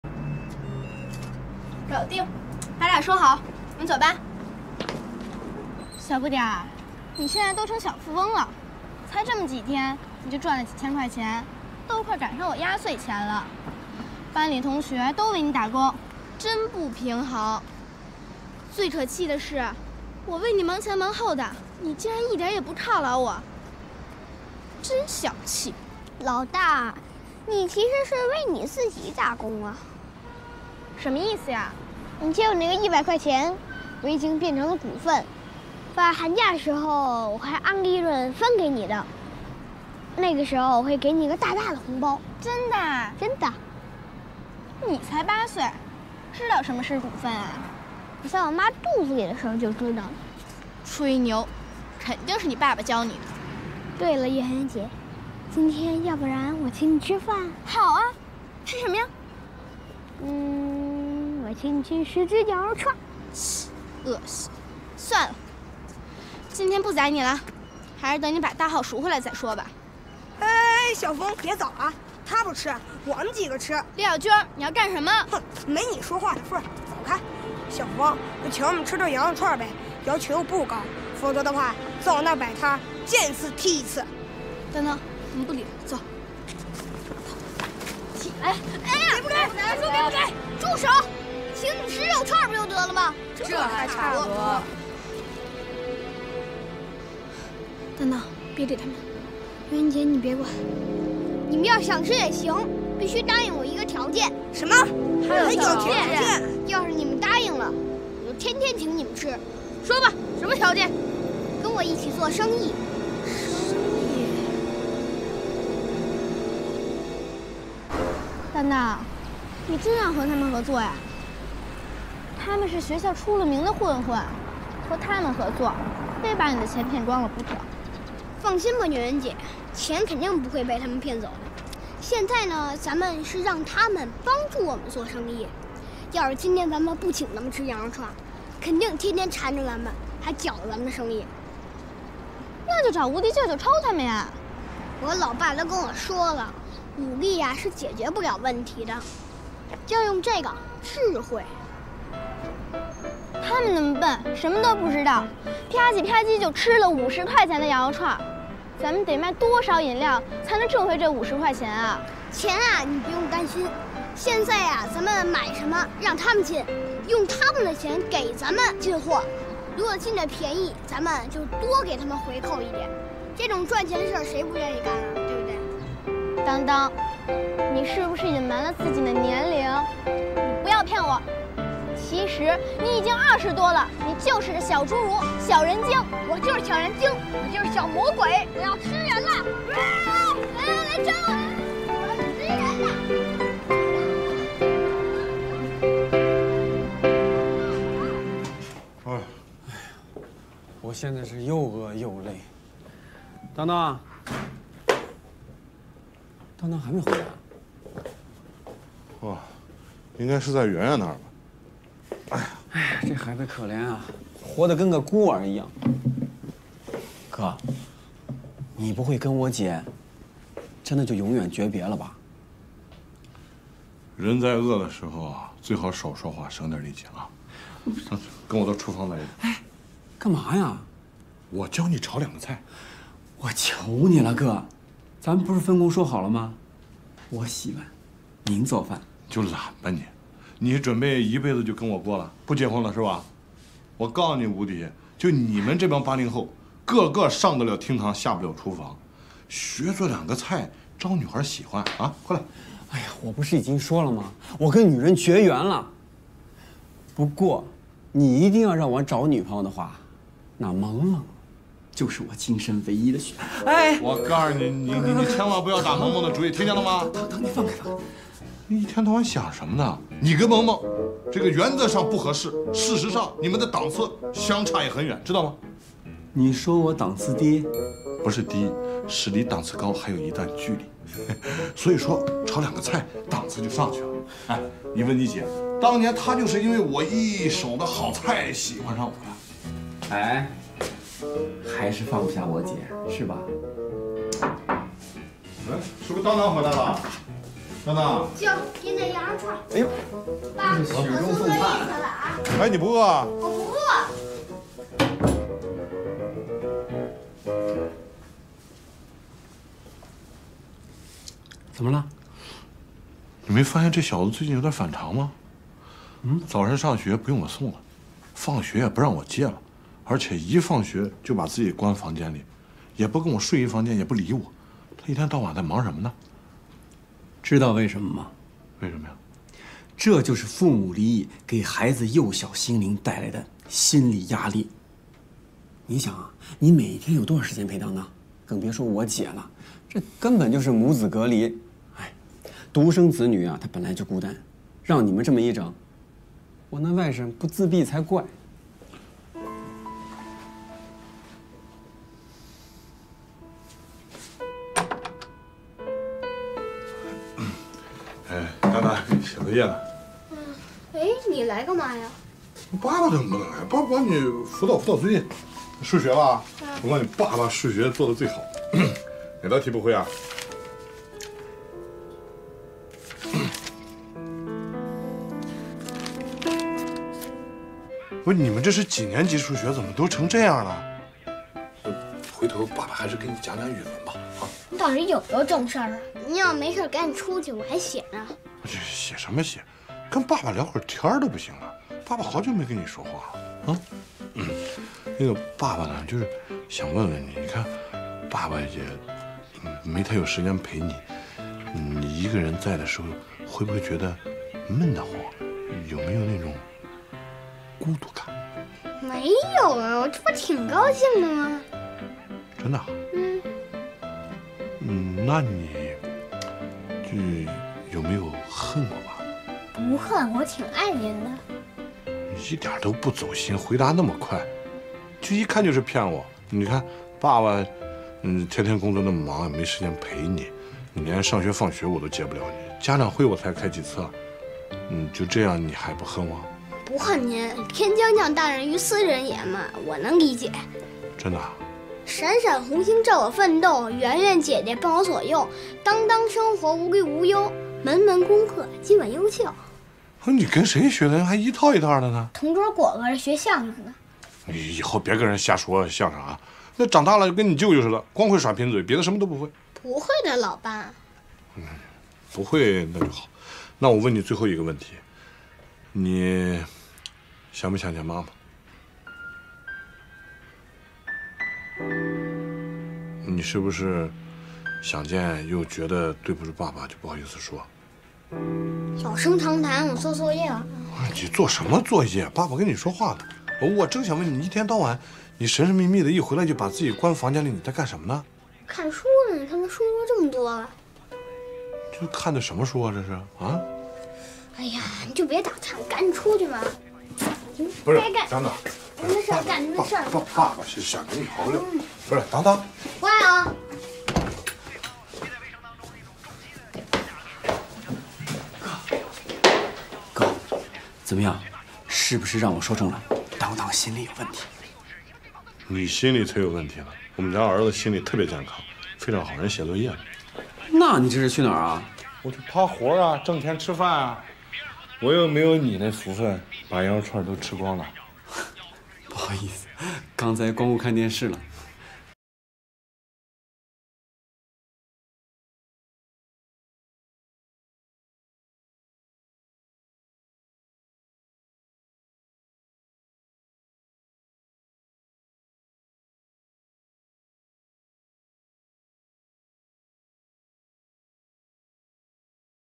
8歲男孩成為大股東，卻被混混盯上，怎料他1招擺平，錢還翻了一番！【10-4】
JYBJJ 30
人到中年的投資公司老總嚴保久迎來了事業的巔峰期，家庭也幸福美滿。 得意的春風刮昏了嚴保久的頭腦，妻子因他不戒烟酒而憤怒出走。 嚴保久融資開發的瑞斯達遊樂場被對手設計陷害，全面失敗落到傾家蕩產，狼狽不堪的他只好帶著兒子當當流落街頭，最終被人逼債，走投無路躲回農村老家。 鄉親的淳樸和商戰中的險惡形成了鮮明的對比，嚴保久决定重整旗鼓，從零開始，最終迎來事業的春天

✨每日更新精彩的中國電視劇
網羅好劇，條條精彩，盡在家庭劇場！